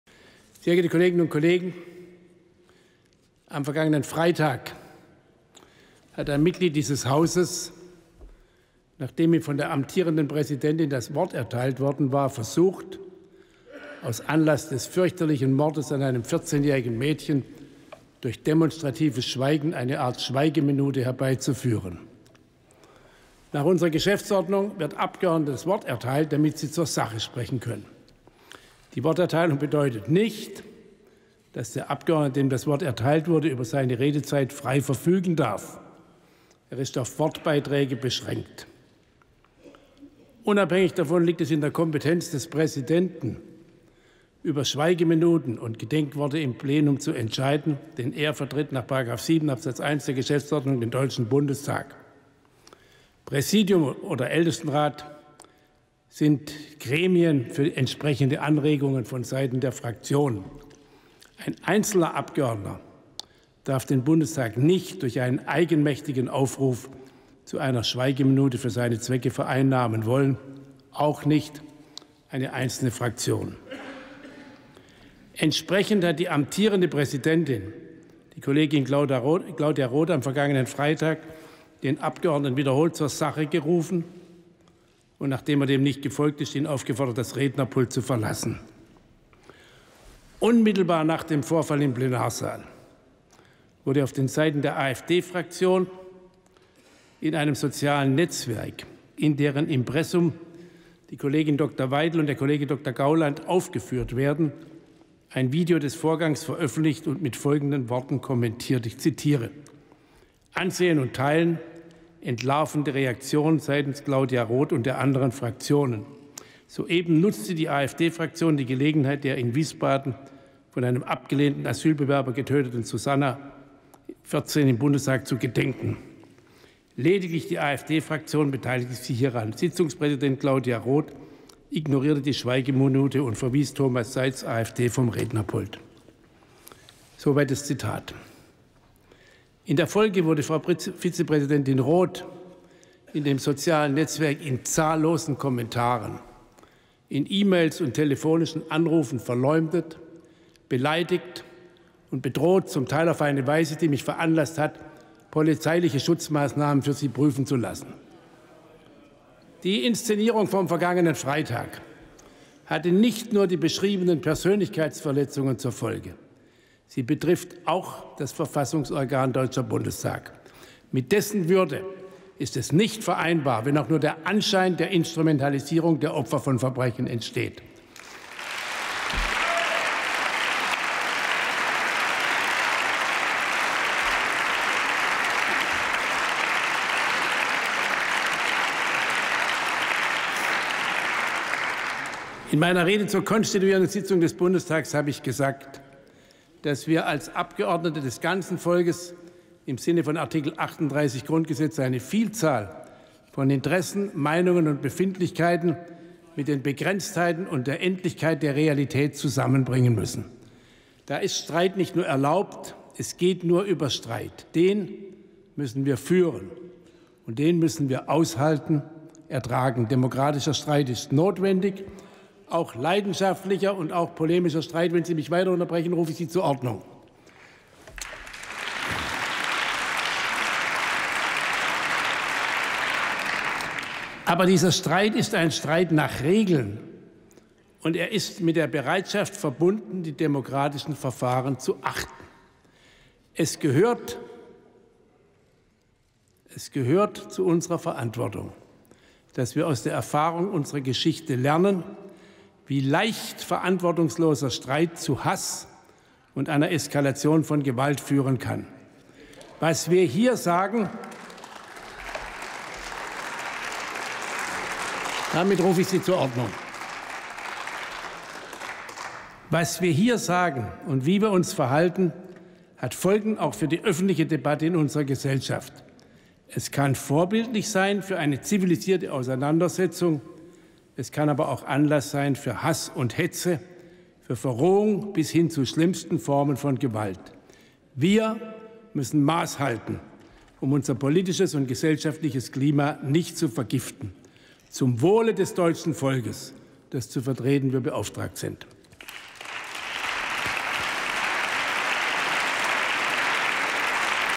Wolfgang Schäuble rügt AfD für inszenierte Schweigeminute
Es ist eine ungewöhnlich scharfe Rüge des Parlamentspräsidenten: Wolfgang Schäuble hat zur Generalansage an die AfD ausgeholt. Anlass ist eine Schweige-Aktion des Abgeordneten Thomas Seitz.